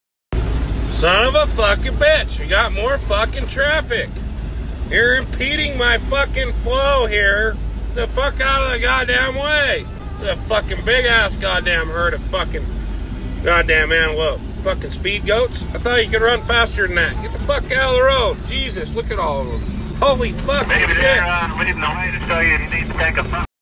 Huge Herd of Antelope on Road in Wyoming || ViralHog
Occurred on February 15, 2020 / Baggs, Wyoming, USA

"I was working driving a semi headed to pick a load up. I was located outside of Baggs Wyoming. These locations are 40 miles of off-road driving. I came upon the herd and started to film. I ended up being able to turn onto a different road."